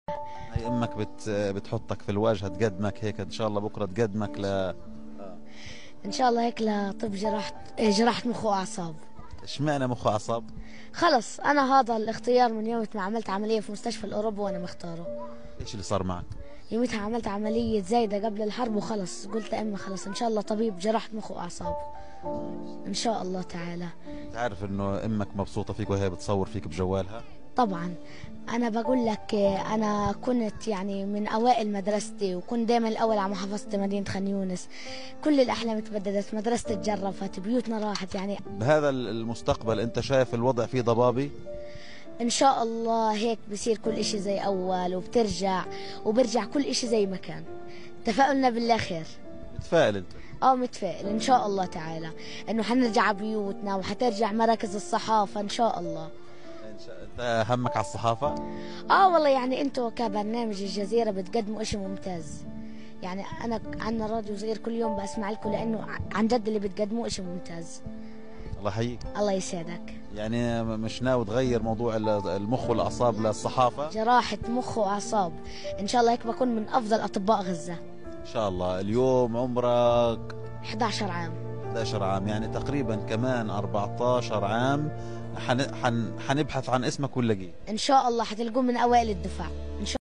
Displaced Gaza child dreams of becoming a neurosurgeon despite war
A displaced 11-year-old boy from Gaza shares his dream of becoming a neurosurgeon. Despite Israel’s ongoing war that has devastated the region, he remains hopeful that things will return to what they once were before the war.